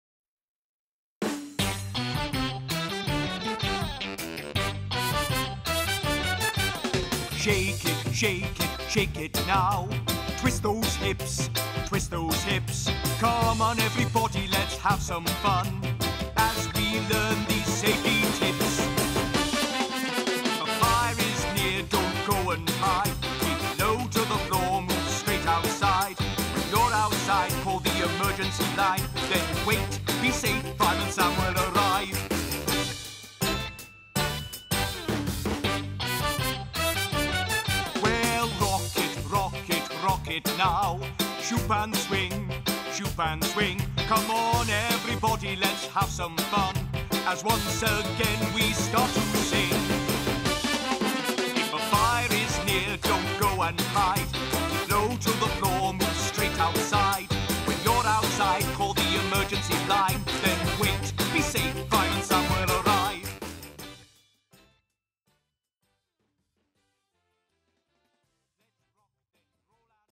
Fireman Sam Saves the Circus | Sun 22 August
Join Sam, Penny, Elvis, Station Officer Steele and Norman in an all singing, dancing, action-packed show. You can become a fire-fighter cadet and then watch the magic of the circus.